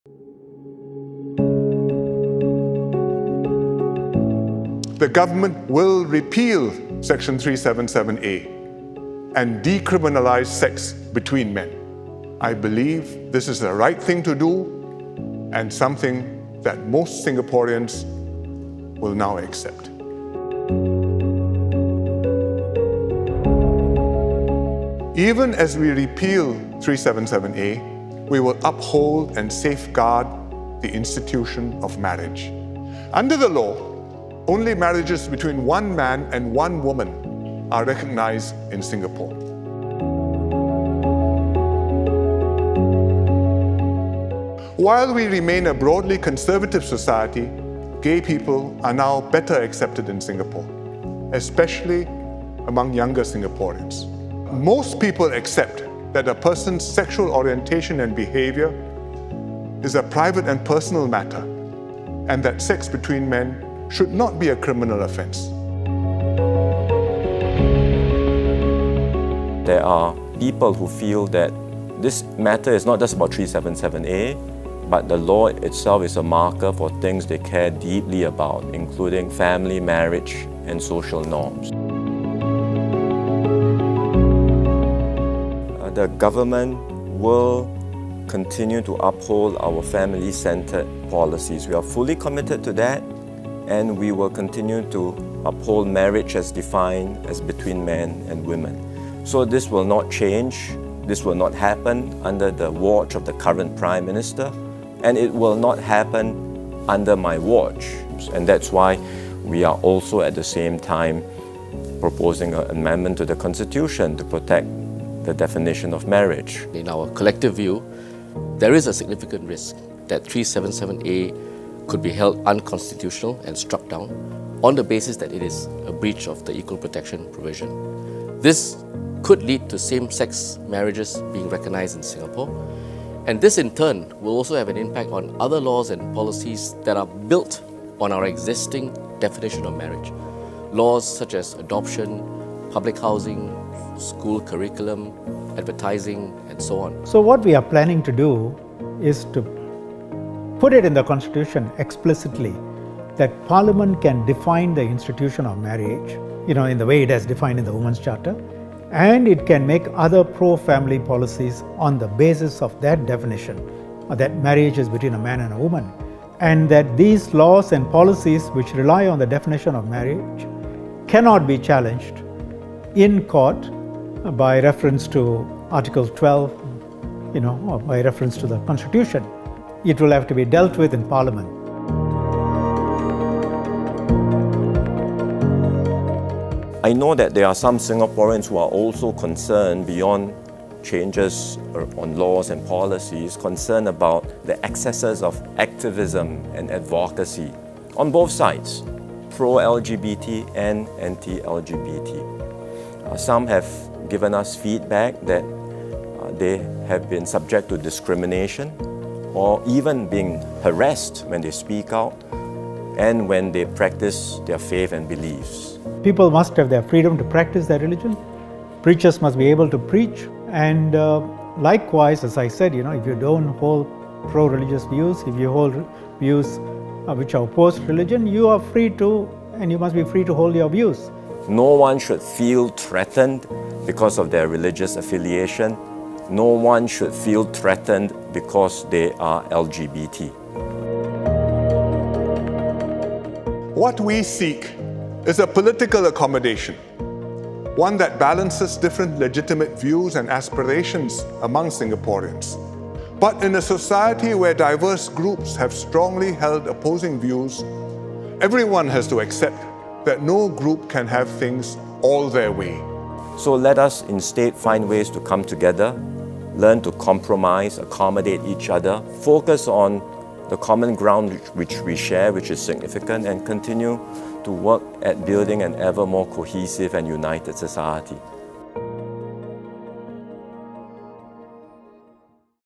Section 377A Explainer: The Government intends to repeal S377A of the Penal Code
The Government intends to repeal S377A of the Penal Code.  It will also amend the Constitution to protect the current definition of marriage.

What do these changes mean, and why is the Government doing this now?